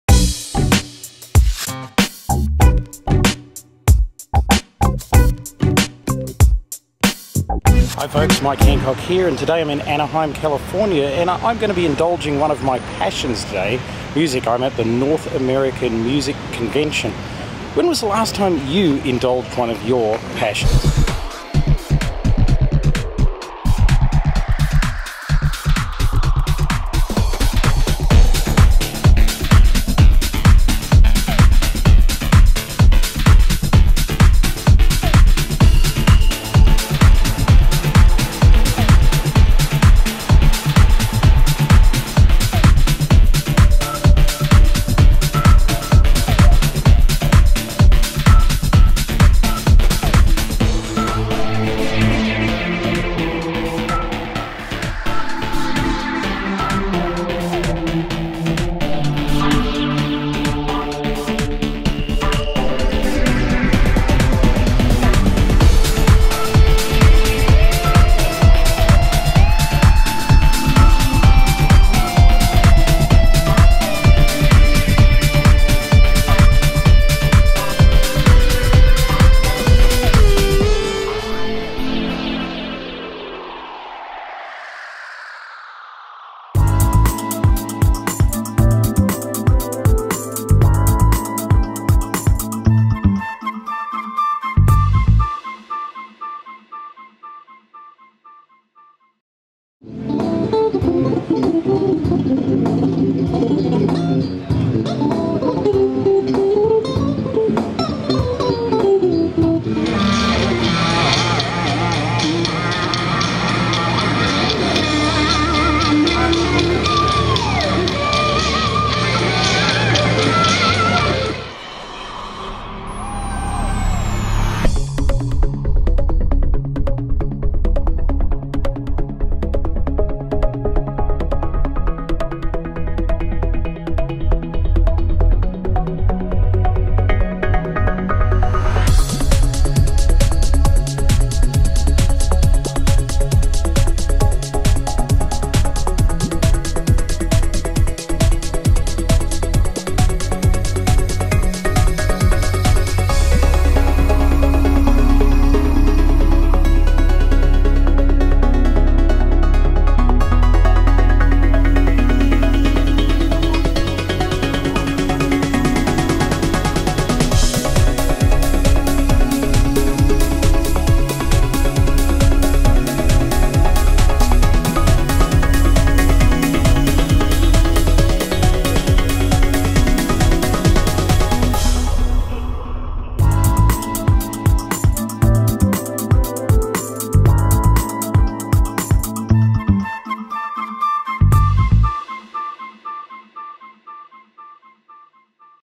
NAMM 2012
Mike Handcock visits the North American Music Convention